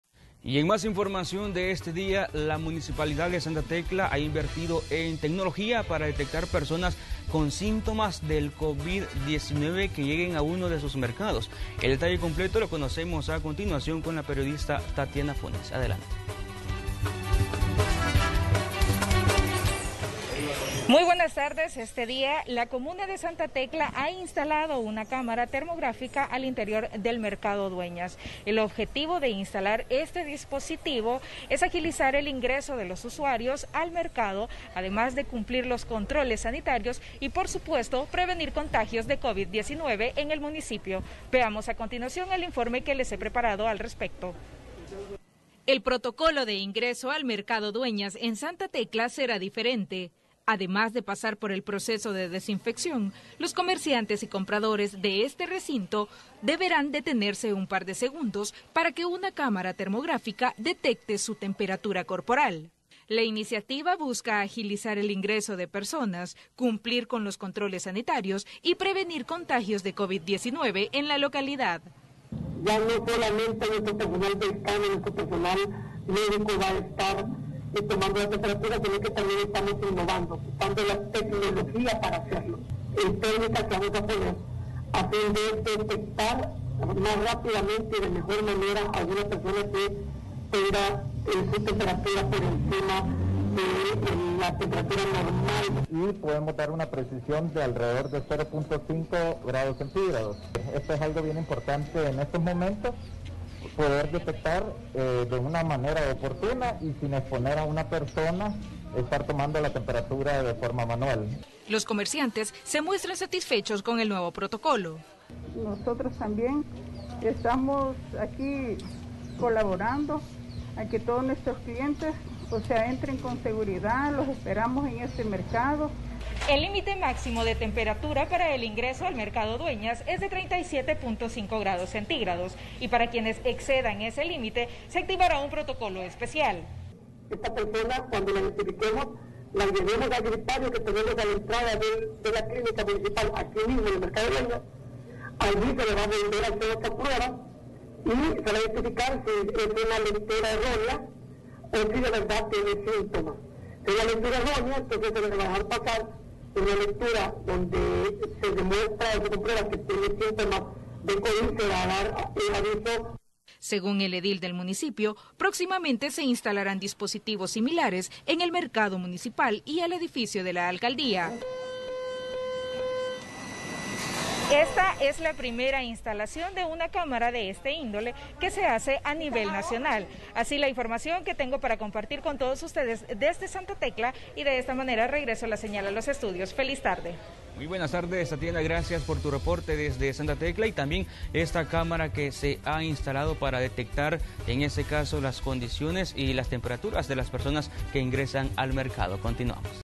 Instalan una cámara termodinámica en el Mercado Dueñas
Esta cámara permite detectar la temperatura de las personas a distancia

La Municipalidad de Santa Tecla ha invertido en nuevas tecnologías, para poder detectar personas con síntomas de COVID-19.